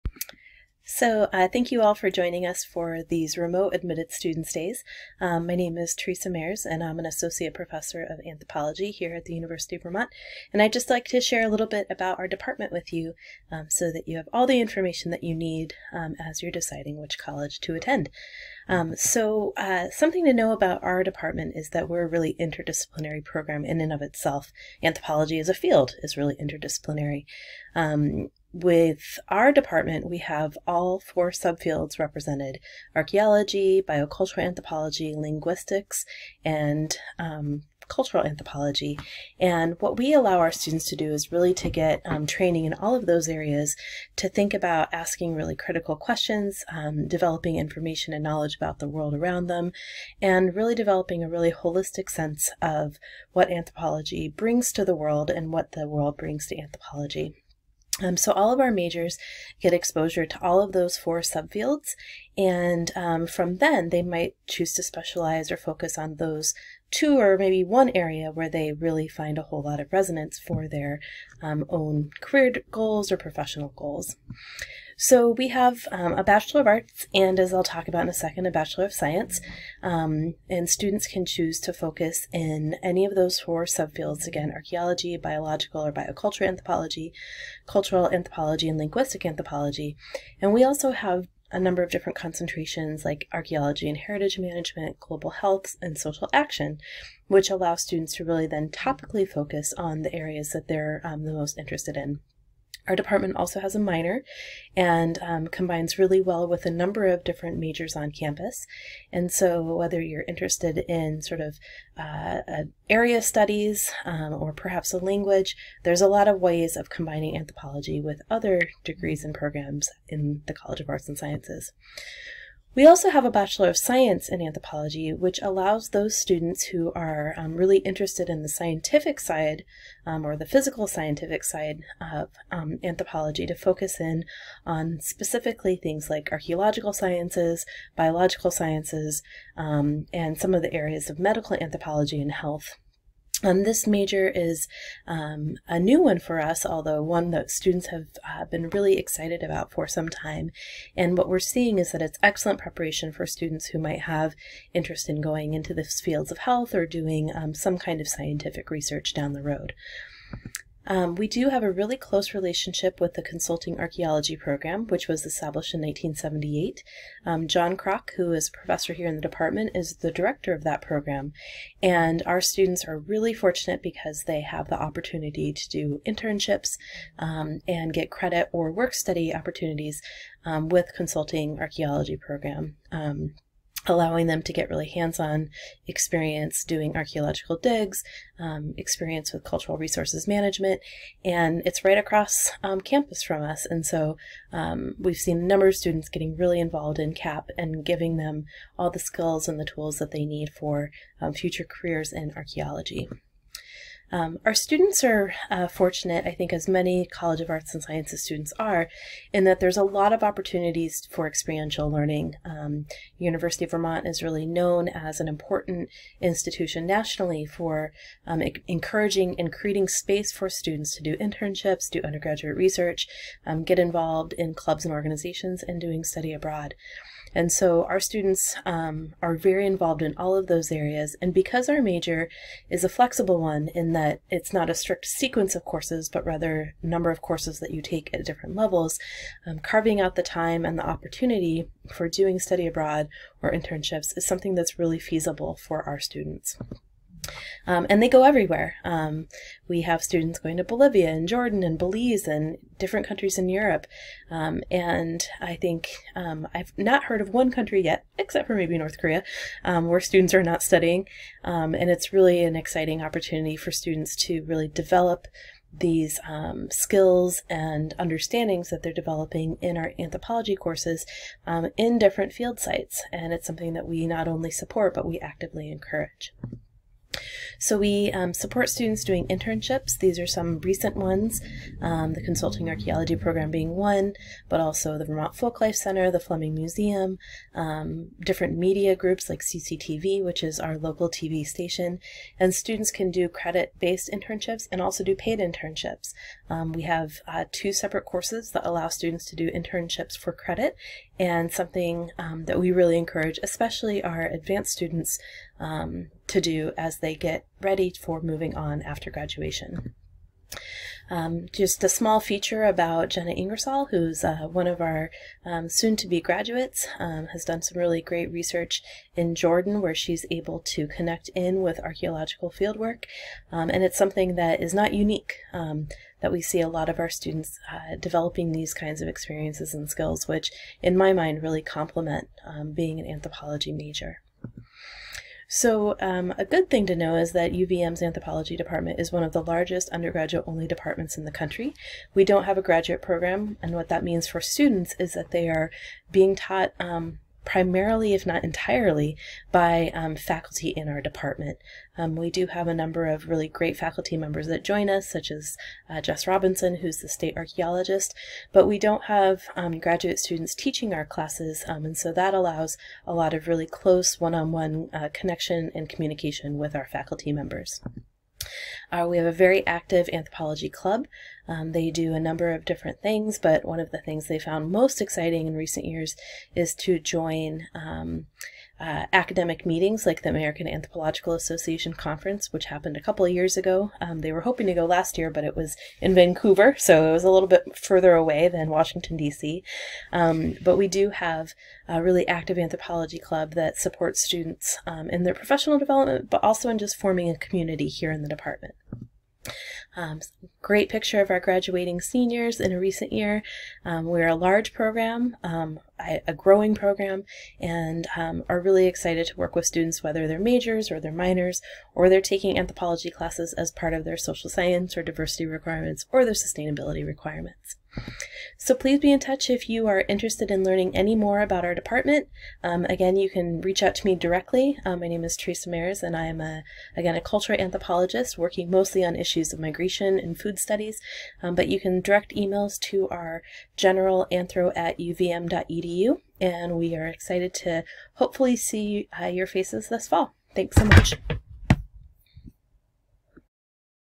AnthropologyASVVideo
Check out this overview of UVM's Anthropology Department narrated by Professor Teresa Mares.